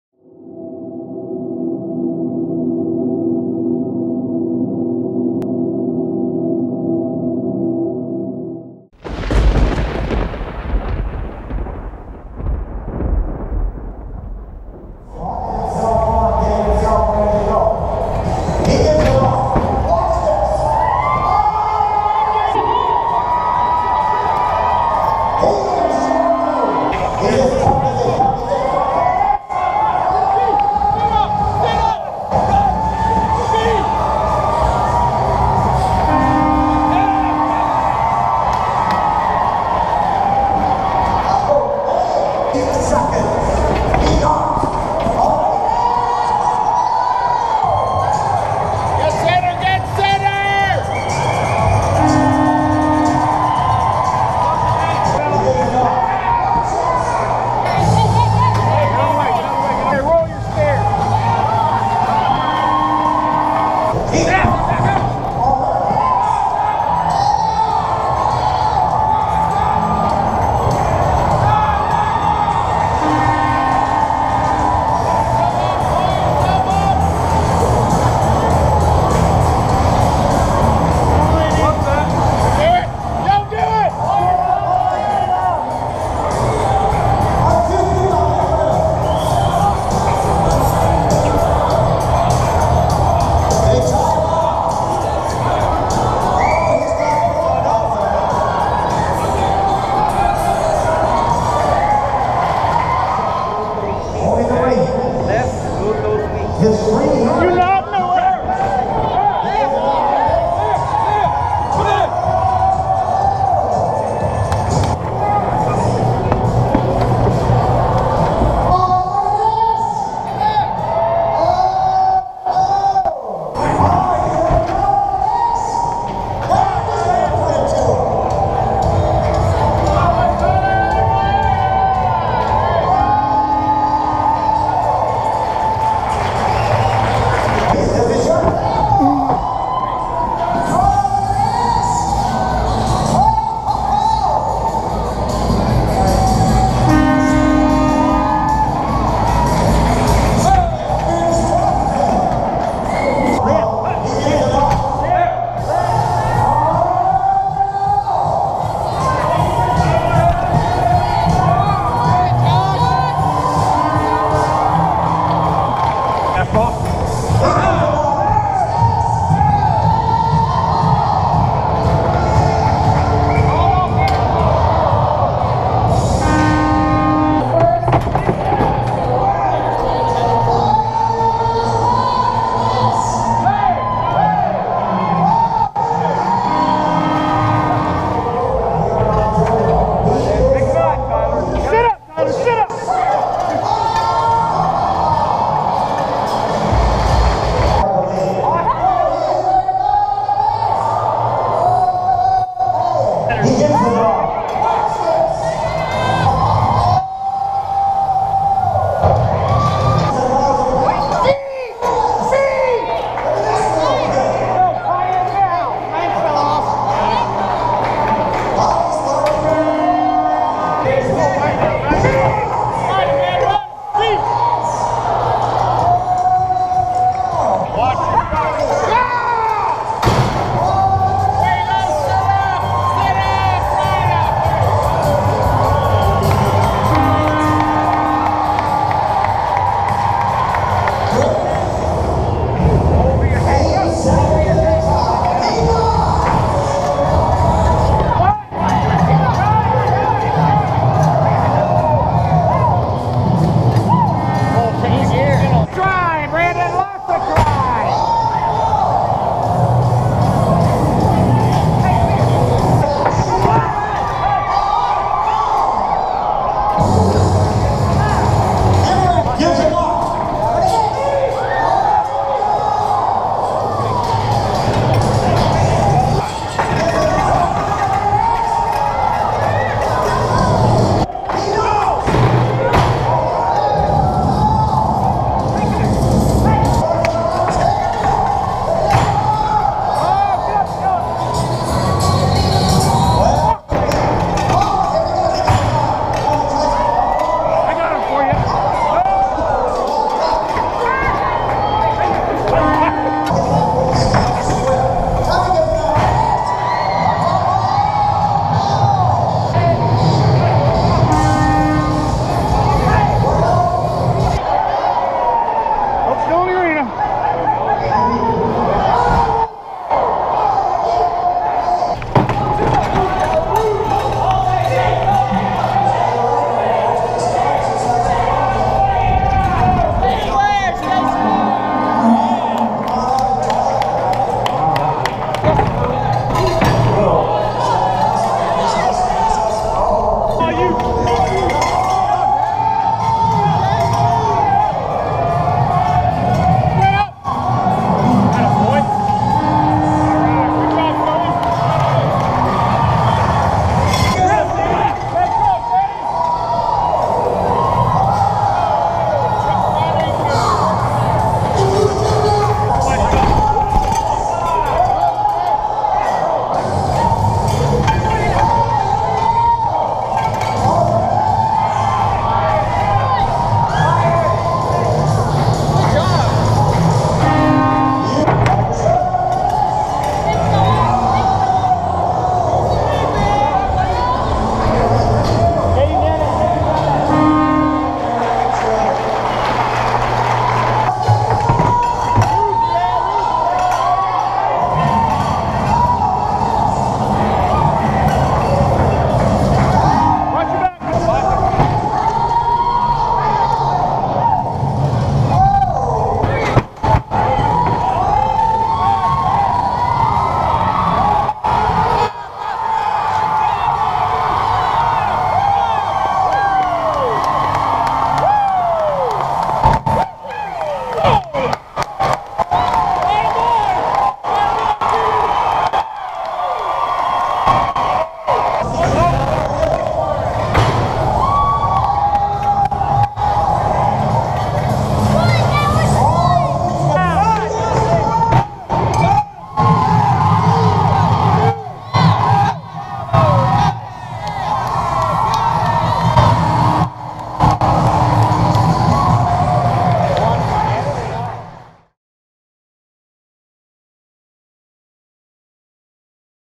New Tecumseth Mayor's Charity Rodeo
A new stop for the CASE IH Ultimate Rodeo Tour  2015 brings us to New Tecumseth ( Alliston Ont) for the Mayors Charity Rodeo .the Alliston  Memorial Arena could be billed as the Alliston house of pain though, be it  rookie or seasoned champ the rough stock can't read the draw sheets and came to buck. With shiny champion belt buckles on the line and qualifying points for the tour championships on the line, the cowboys and cowgirls let it all hang out to make the 8.